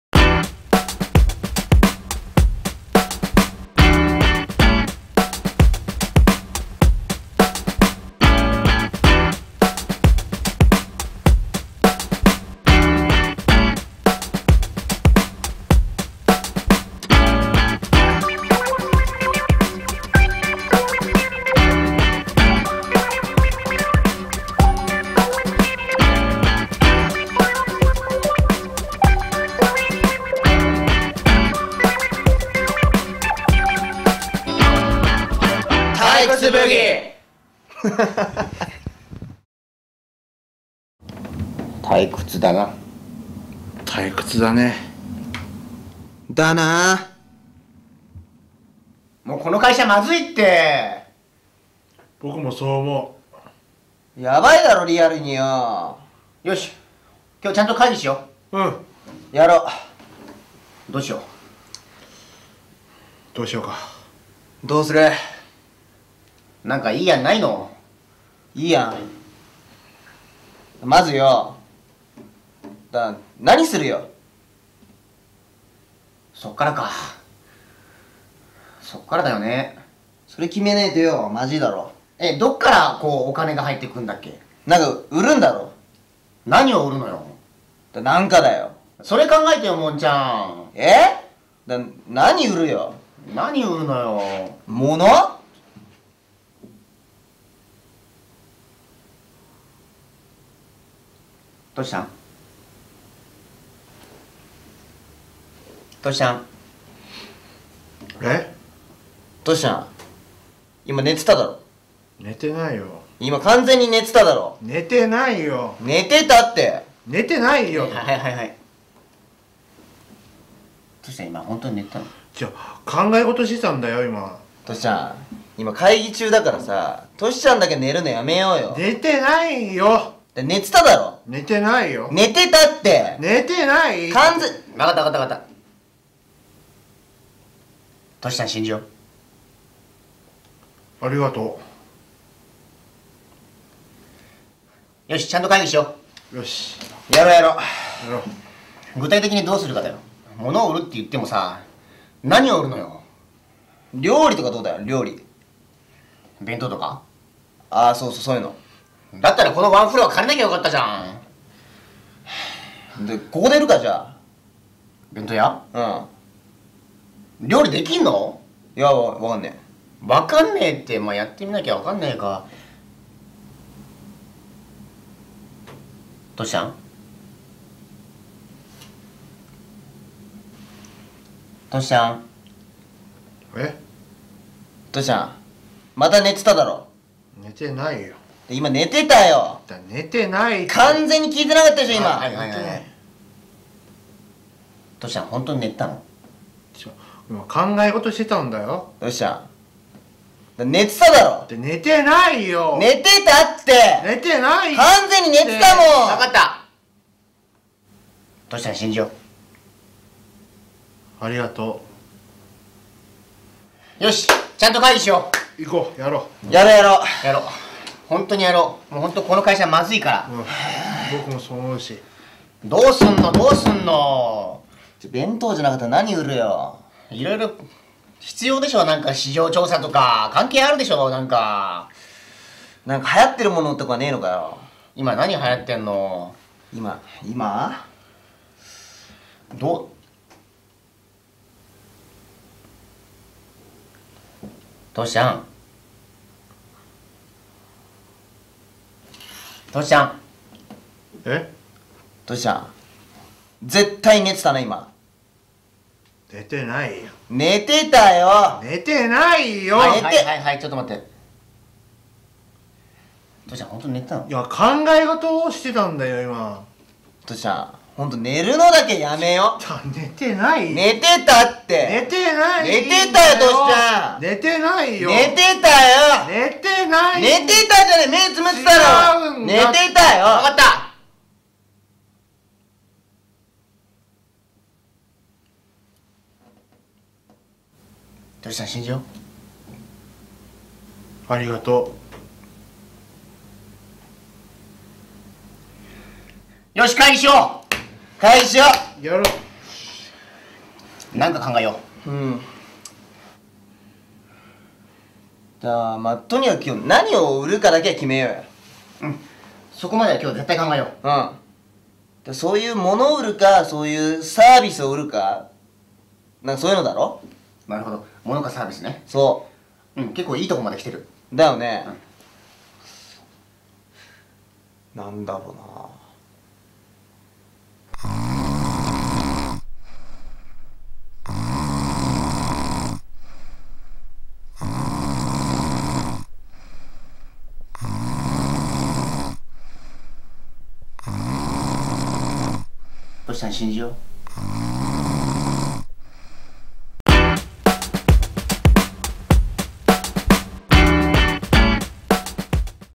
【痛快！ゆるゆるショートV”退屈★ブギ”】第２話：会議
もんどからコメントが来た！うれしいー！
あ、はい。こちら昔、作ったショートストーリー動画です。退屈から生まれるストーリーなので今の時期にぴったりの話かなと。ちなみに、このシリーズ２０話くらいまであります。。。

とにかく、テーマは”ゆる〜い脱力系のショートネタ”です。
あ！映像が荒いです。ご了承ください。

まだまだありますのでまたアップしていきます。
おうち時間に笑いを！

▼デフロスターズHP（しばらく更新してませんが）はこちら